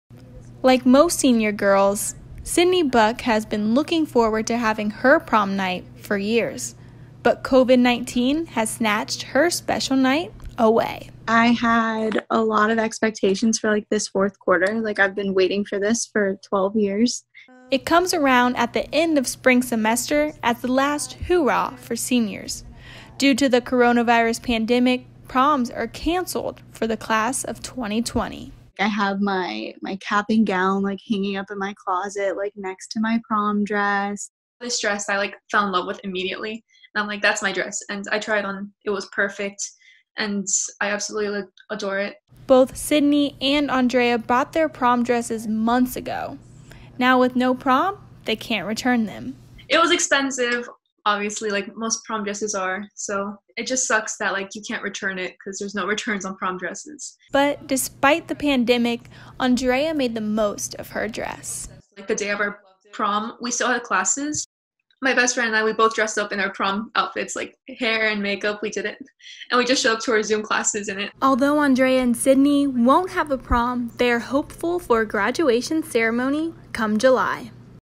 Jillian Oddo reports on Senior Proms being cancelled.
Prom night snatched away from the class of 2020. For those that already bought their prom dresses, they can't return them.